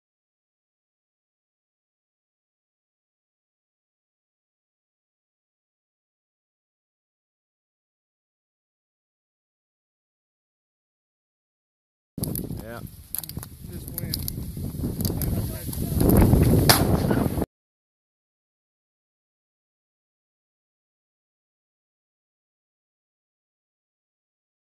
Mark, Paul, Jordan, Jon, Joel and Jacob Pheasant and Chukar Hunt
Mark, Jordan and Jacob Ing came out for a hunt with their friends, Paul, Jon and Joel Witt.  Mark brought out his new lab pup, Aela, to meet some new people and smell some new smells.  It was warm and windy and the group wanted to hunt for pheasant and chukar so I stocked the north end of the Tinkerfarm Field and Greg Ireland brought out Daisy to do the guiding.  Daisy, just off the disabled list, did a great job finding the birds in the milo strips and the group came back shy of just a few birds.